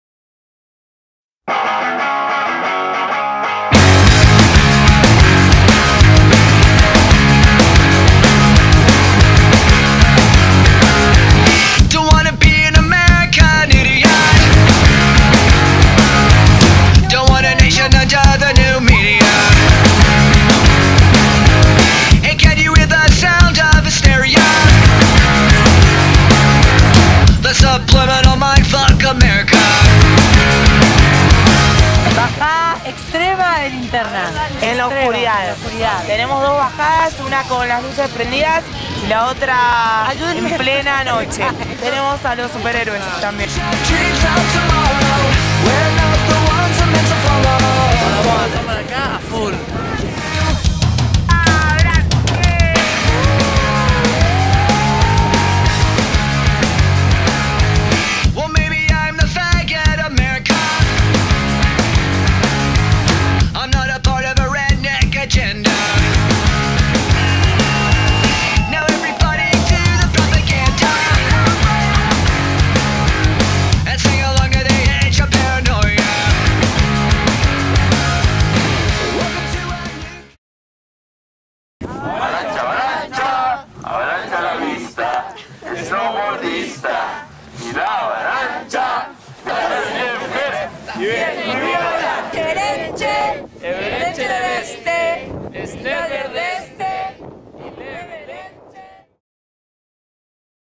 Bajada de Linternas
Todos los jueves nuestros Olimpos realizan la tradicional bajada de linternas en la pista Eros I